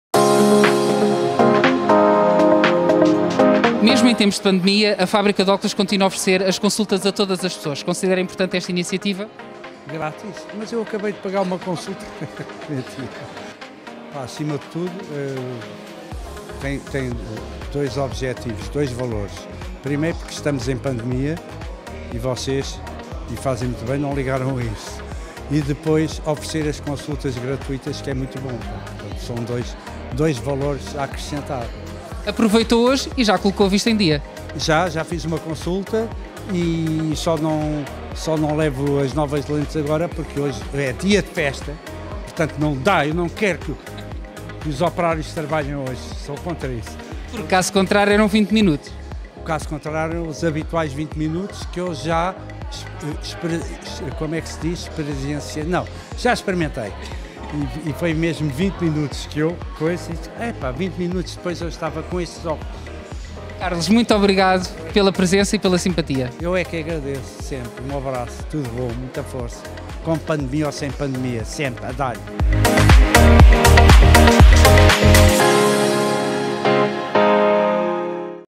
Carlos Areia elogia vantagem importante da nossa ótica durante a pandemia!
Num novo ano, o ator continua a confiar a sua visão nossa ótica!